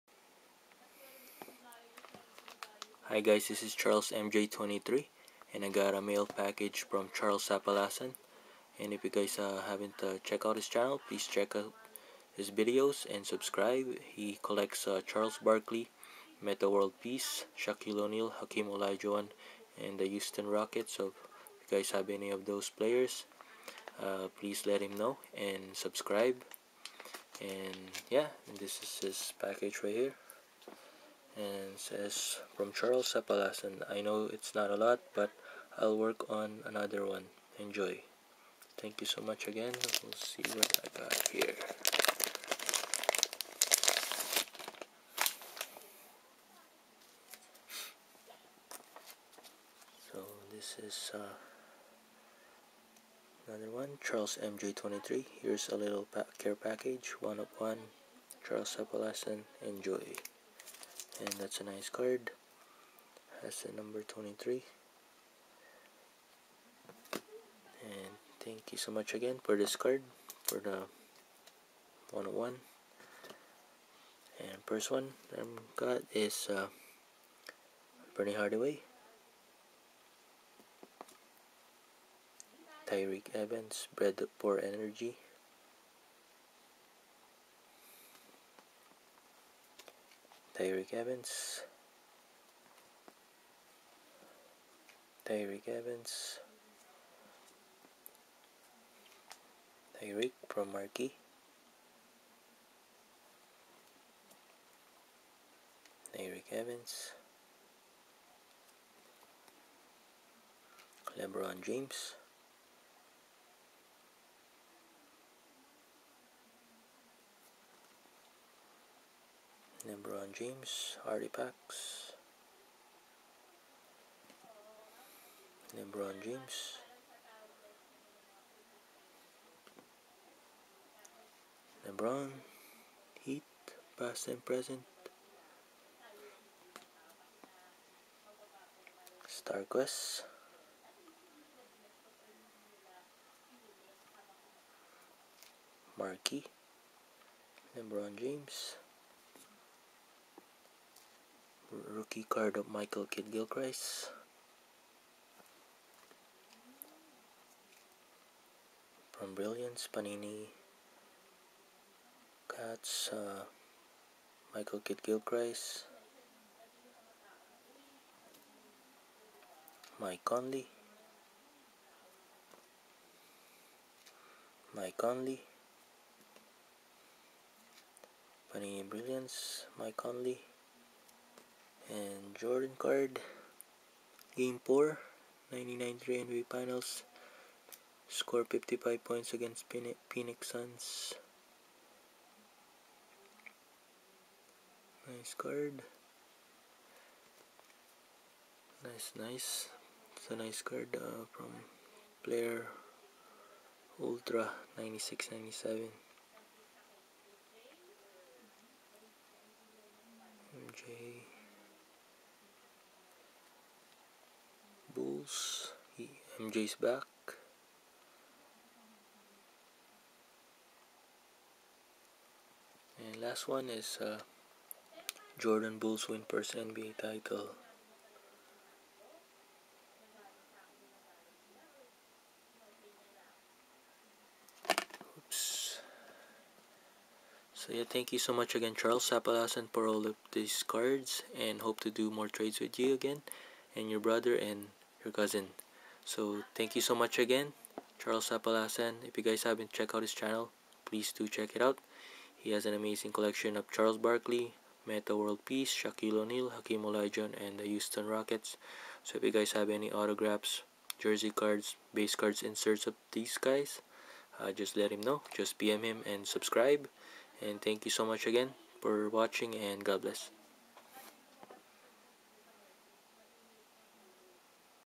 Package Mail Day From Charles Sapalasan - 10/14/13 AWESOME PACKAGE CHARLES!!! SUBSCRIBED!!!
Thank You Very Much Charles Sapalasan!!! for the trade & I hope we can do more trades in the future. Thank You!!! for helping my Michael Jordan PC, God Bless bro & take care.

SUB = ADD = LIKED = Charles Sapalasan

Thank You Very Much Charles Sapalasan!!! God Bless!!!